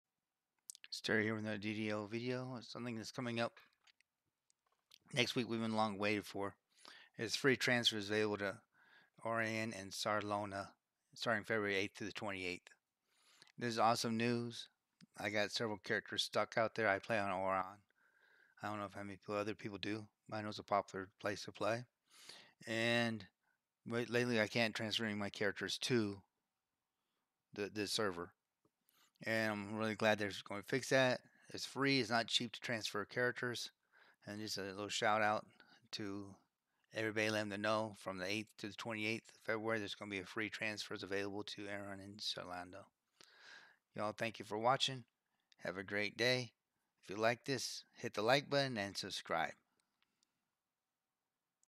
Dungeons & Dragons Online DDO Free transfers to Orien and Sarlona
Short Video going over free transfers to Orien and Sarlona from 8 FEB to 28 FEB 2023